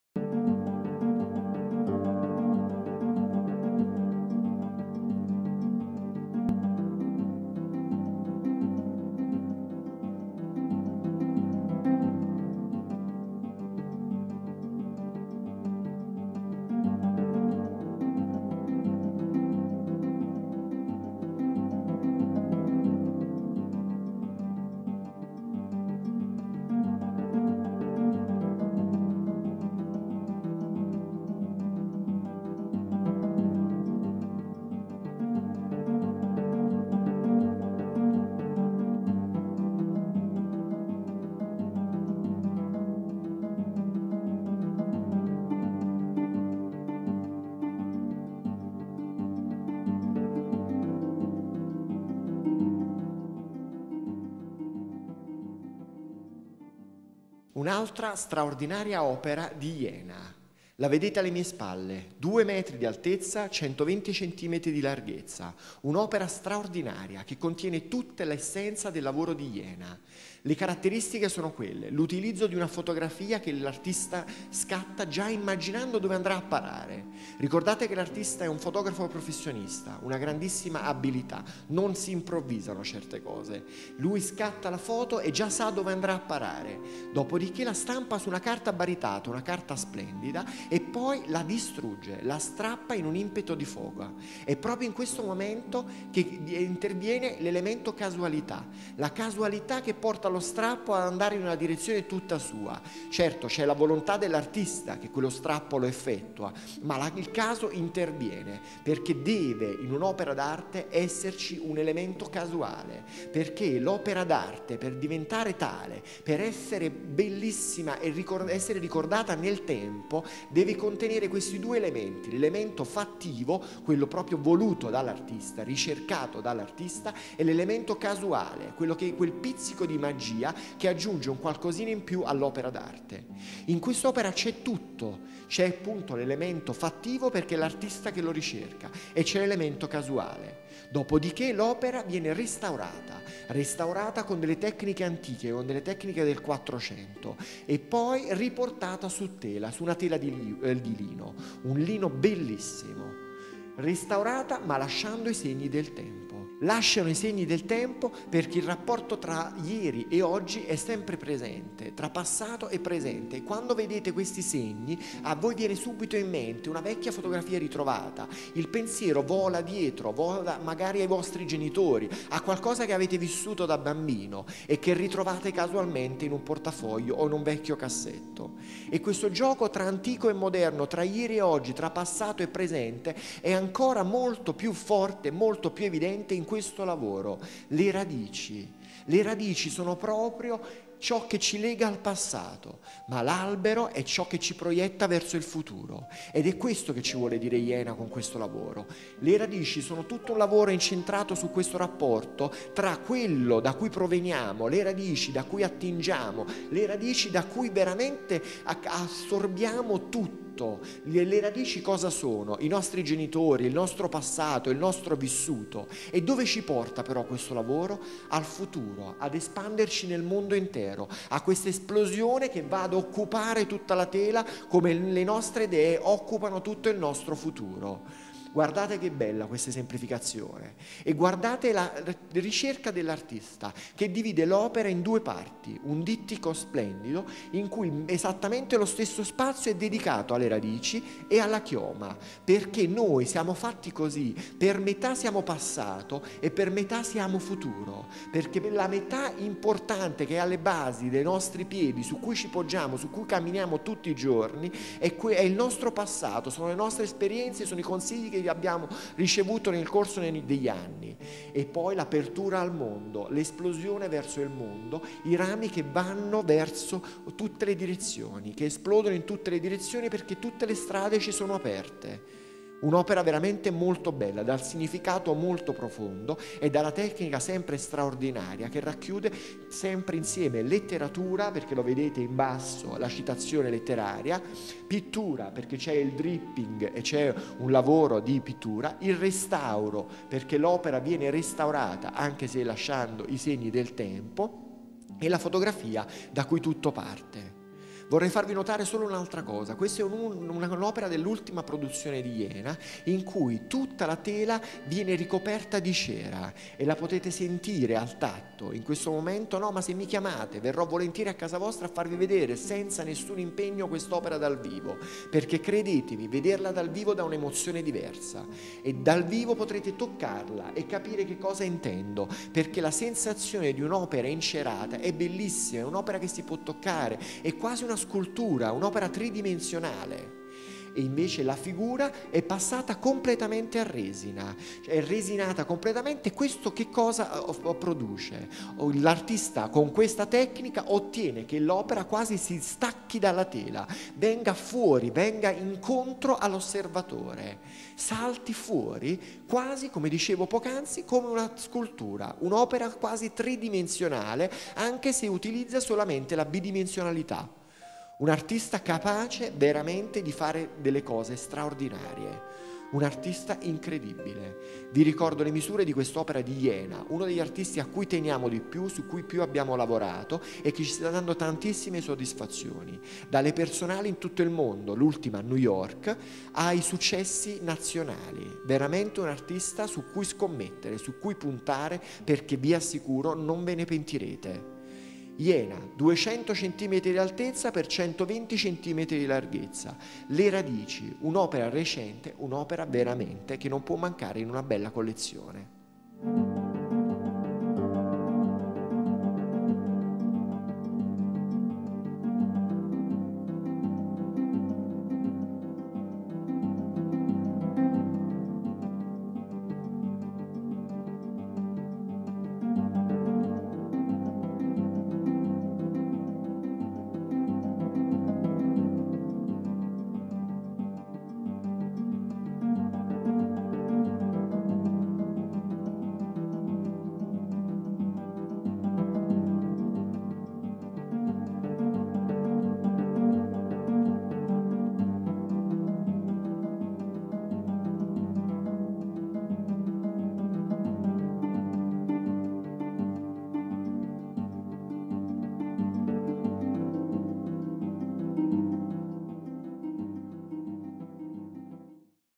Hyena, Radici, Galleria de' Bonis
Hyena, Radici, tecnica mistra su tela di lino, cm. 180 x 130
In vendita presso Galleria de' Bonis, Reggio Emilia, Viale dei Mille, 44/B.